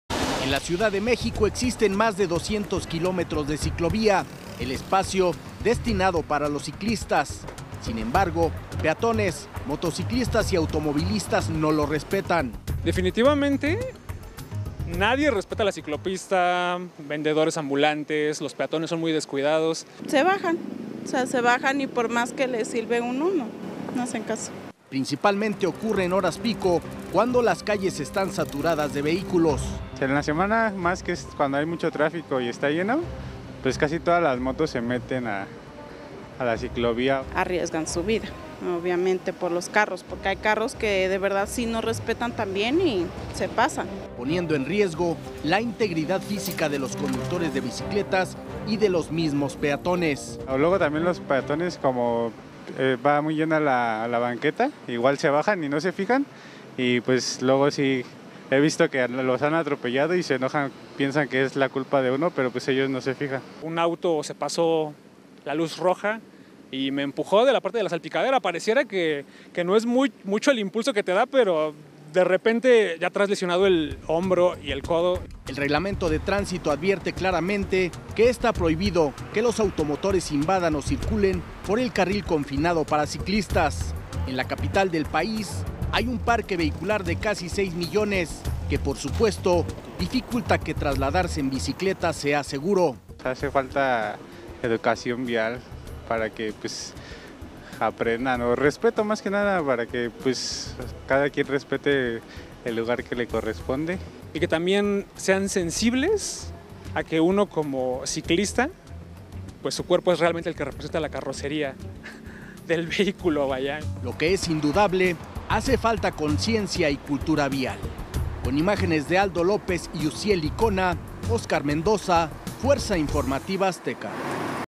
La hora pico en CDMX, se convierte en un peligro para los ciclistas: ¡ni los peatones respetan!
CDMX | Nadie respeta las ciclopistas; ponen en riesgo a los conductores de bicicletas y peatones.

En la capital del país hay más de 200 kilómetros de ciclovía, un espacio destinado para los ciclistas. Sin embargo, nadie respeta el espacio; automovilistas, motos y hasta peatones invaden el lugar.

Principalmente, ocurre en horas pico, ya que las vialidades están invadidas por autos. Ciclistas piden cultura vial y respeto para ellos.

El reglamento de tránsito advierte que los automotores invadan los carriles confinados para ciclistas.

Con información de Óscar Mendoza